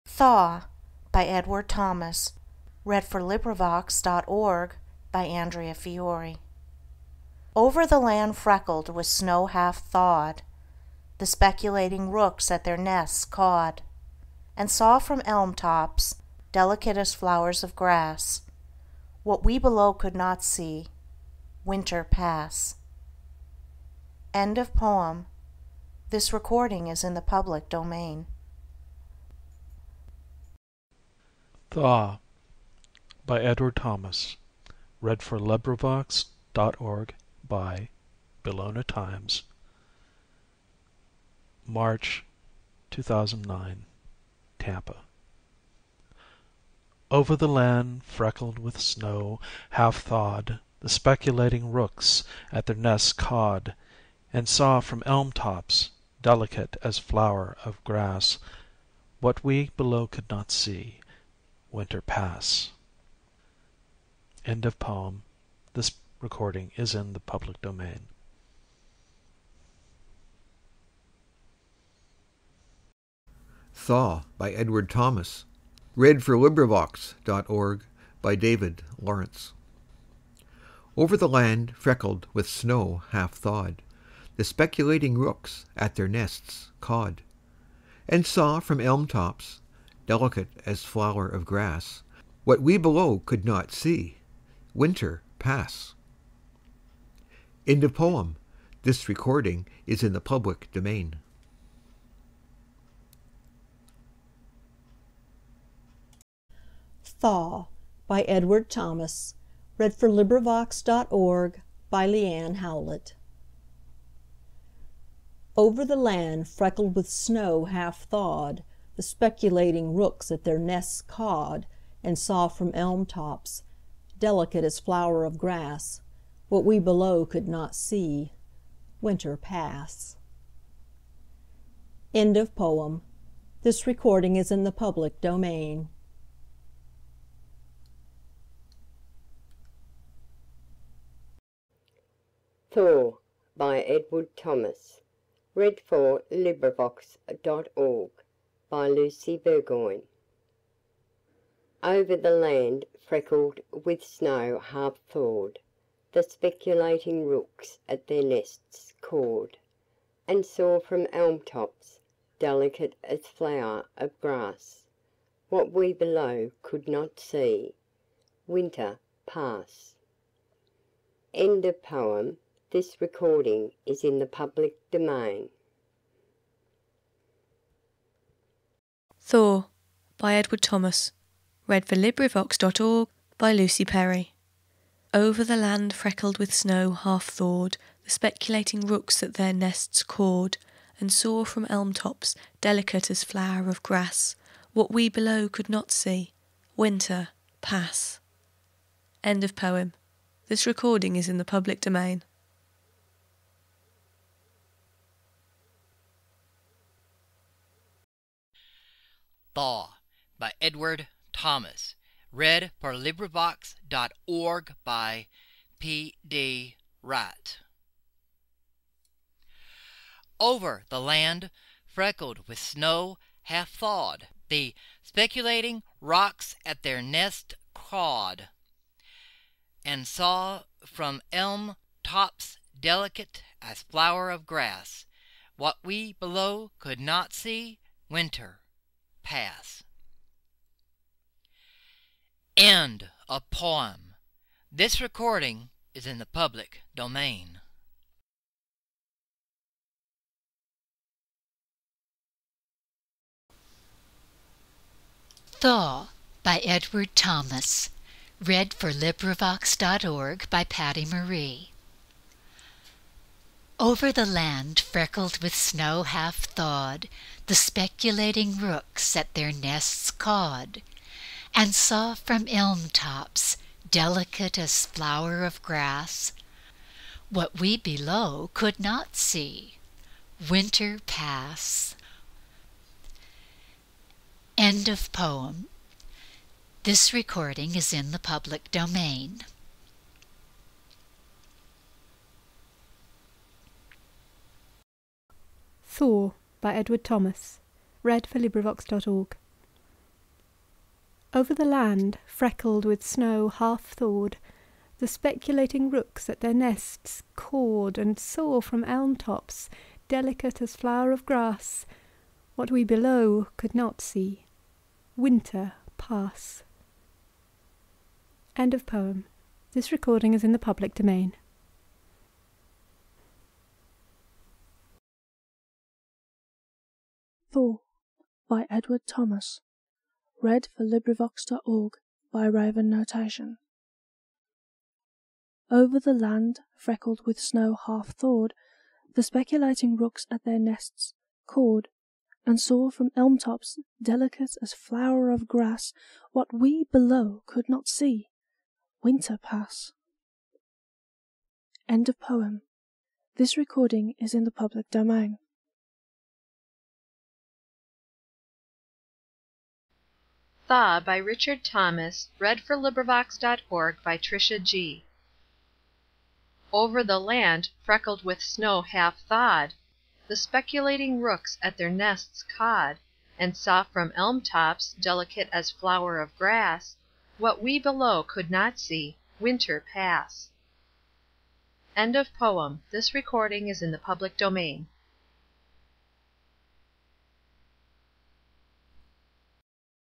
Thaw by Edward THOMAS read by Various | Full Audio Book
Thaw by Edward THOMAS (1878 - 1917)
Genre(s): Multi-version (Weekly and Fortnightly poetry)

Read by: Andrea Fiore, Bellona Times, David Lawrence, Lee Ann Howlett, Lucy Burgoyne (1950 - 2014), Lucy Perry, P. D. Wright, pattymarie, Philippa, ravenotation, TriciaG in English

Chapters:
00:00:00 - 01 - Version 1
00:00:30 - 02 - Version 2
00:01:12 - 03 - Version 3
00:01:45 - 04 - Version 4
00:02:20 - 05 - Version 5
00:02:57 - 06 - Version 6
00:03:26 - 07 - Version 7
00:04:11 - 08 - Version 8
00:04:53 - 09 - Version 9
00:05:22 - 10 - Version 10
00:05:57 - 11 - Version 11

LibriVox volunteers bring you 11 recordings of Thaw by Edward Thomas. This was the weekly poetry project for March 1st, 2009.